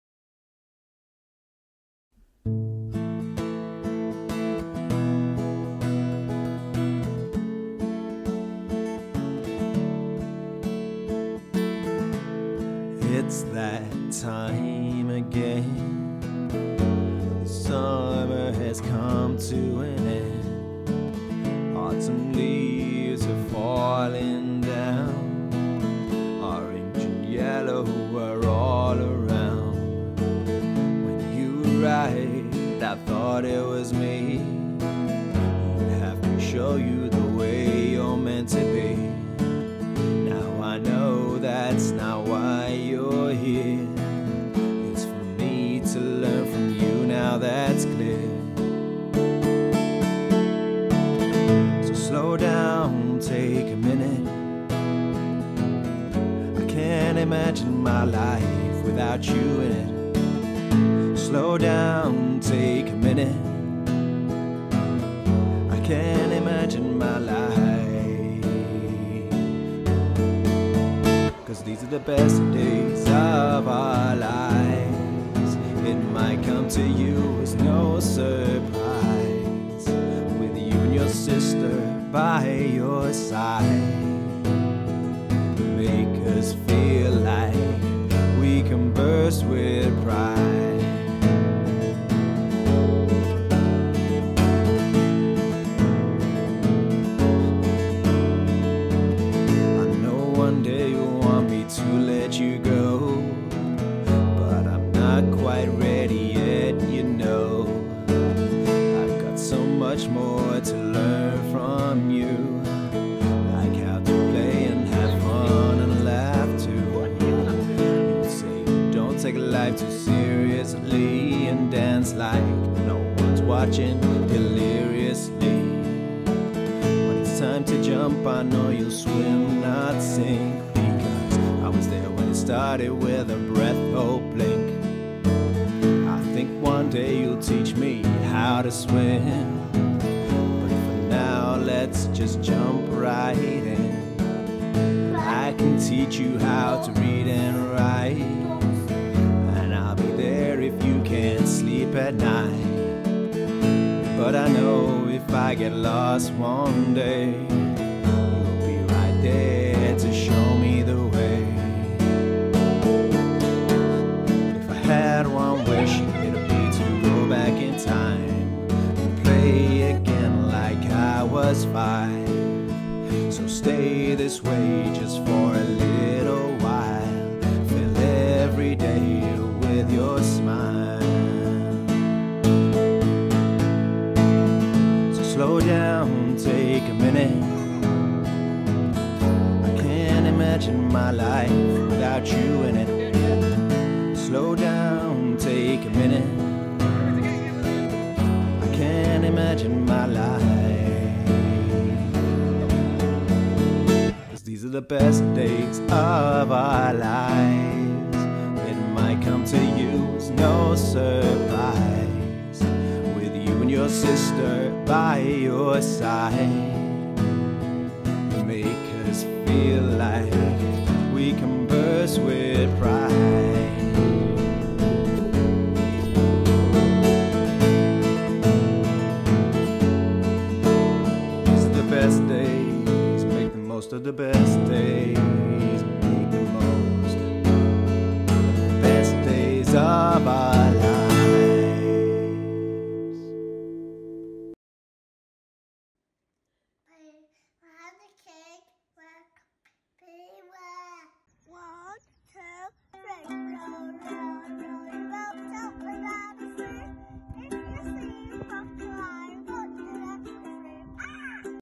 Best Days of Our Lives - Happy 5th Birthday William
Today our kind, loving and gorgeous son (inside and out!) turns 5. This short video is a selection of some of the times we have shared and only touches the surface of the experiences and joy that William has brought to all of our lives!